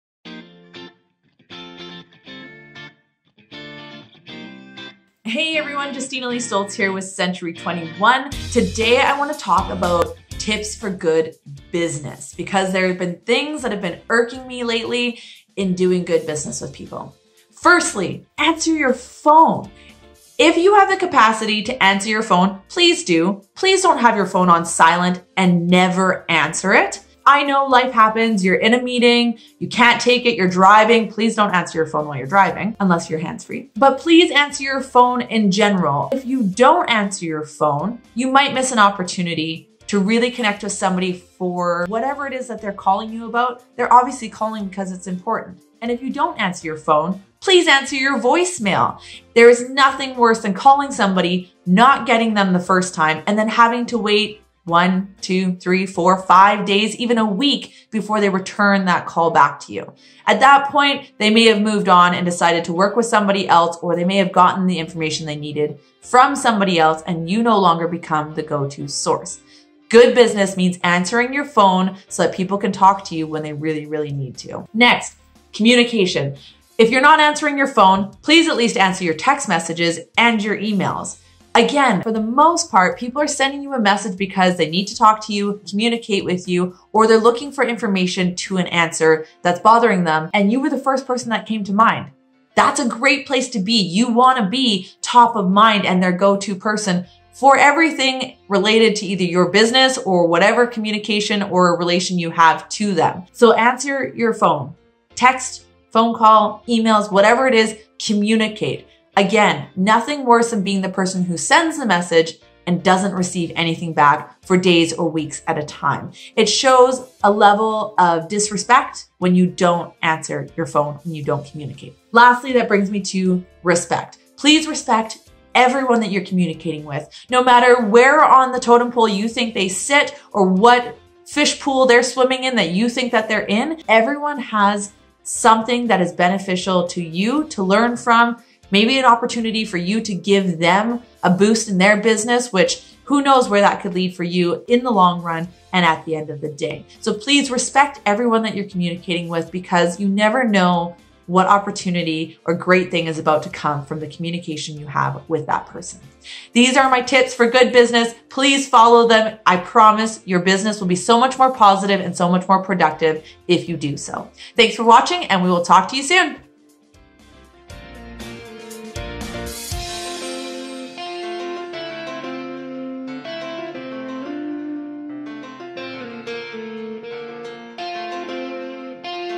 VLOG Episode 5: Tips for Good Business
Communication is one of the keys to success no matter what business you are in. What better way to communicate than to  answer your phone, return your messages and respect everyone you are interacting with?

These 3 tips have been crucial in my continued success and are emulated by the most successful people that I follow.

Hope that these will help you too!

Keeping you connected,
Justina